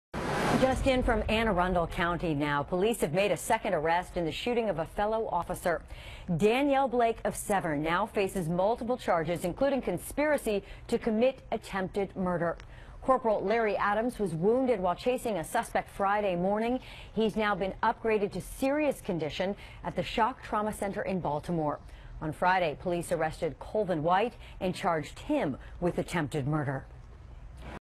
2nd suspect held in Anne Arundel officer's shooting, police say
Anne Arundel County police say a second suspect has been arrested in the shooting of a police officer who remains in serious condition.